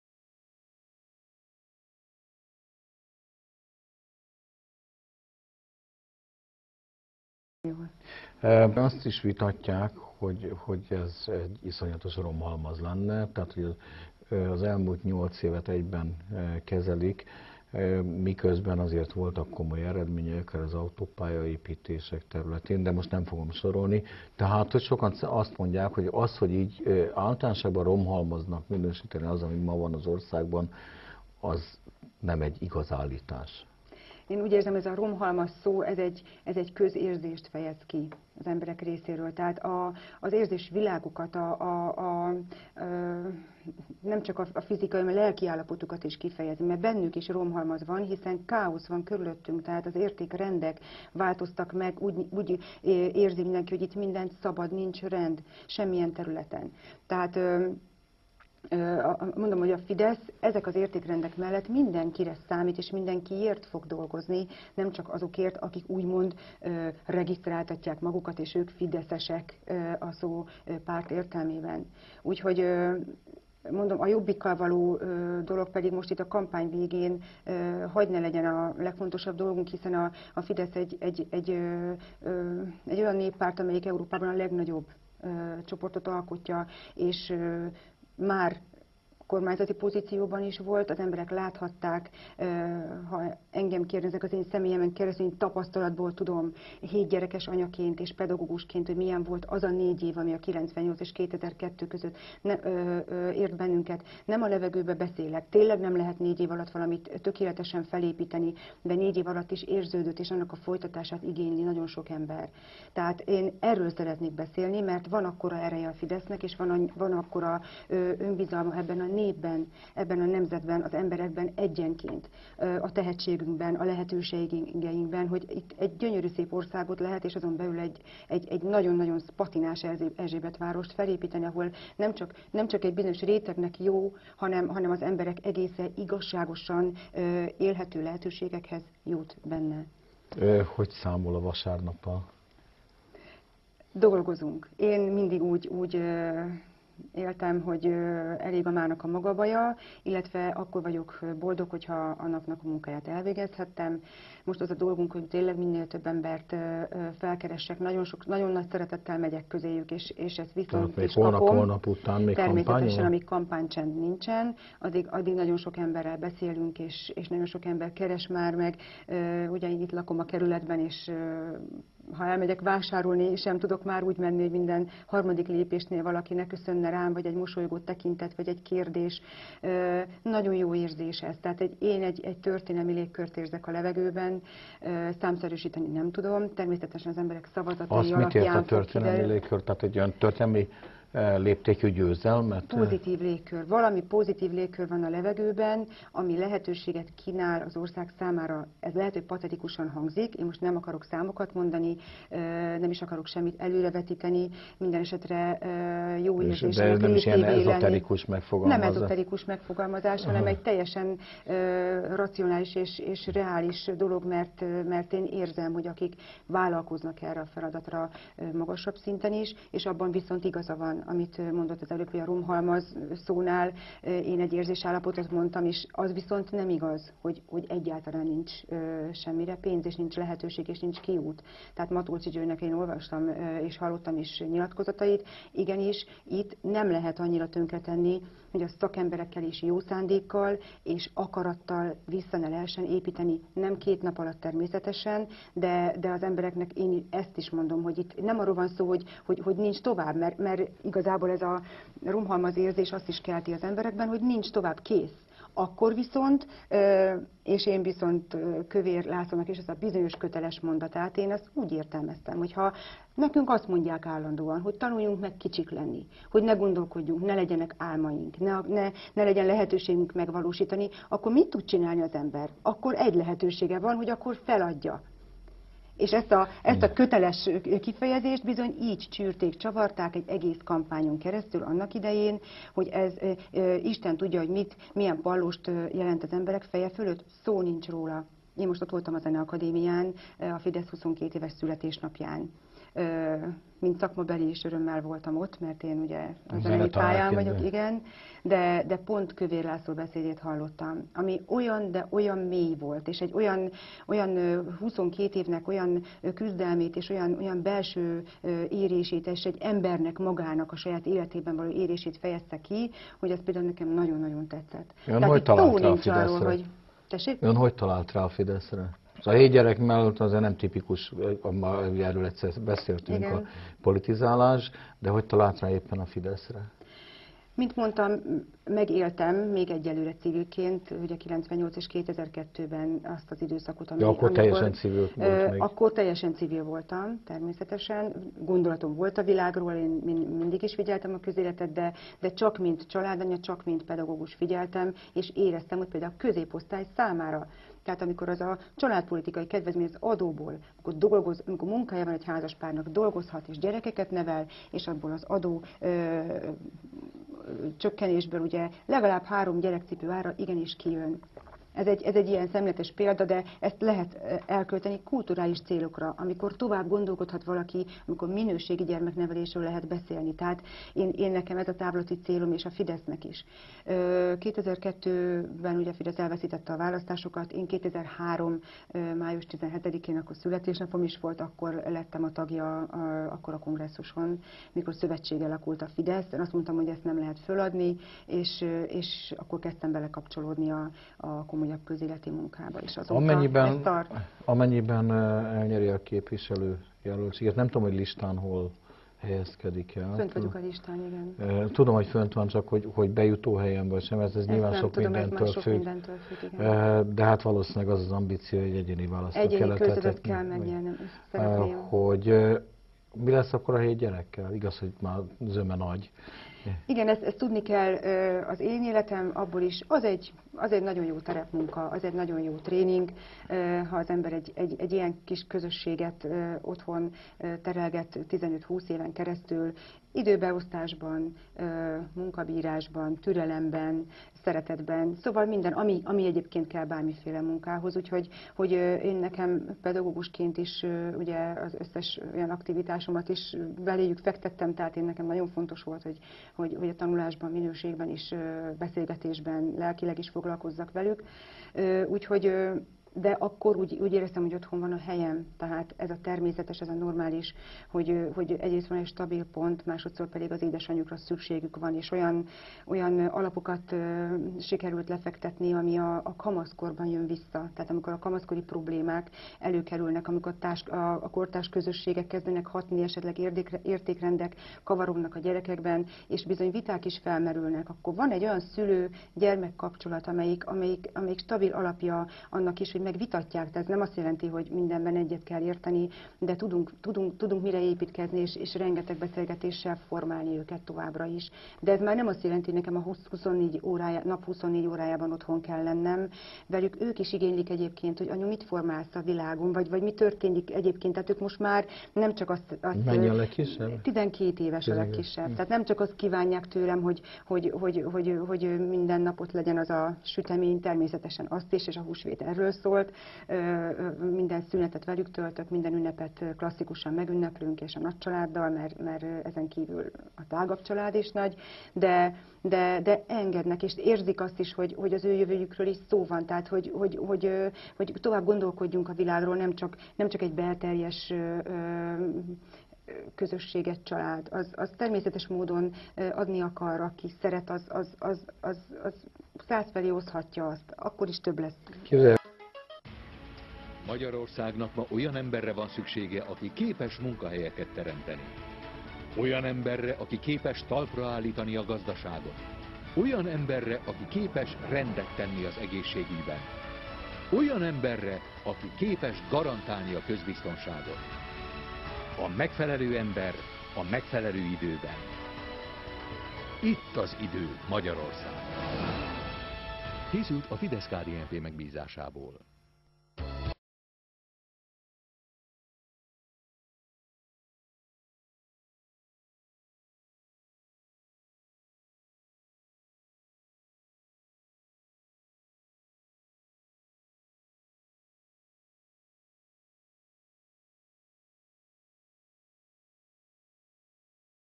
ETV 2. rész.wmv
Beszélgetés Rónaszékiné Keresztes Monikával, a Fidesz - KDNP országgyűlési képviselőjelöltjével az ETV-ben. Riporter: Németh Péter
2. rész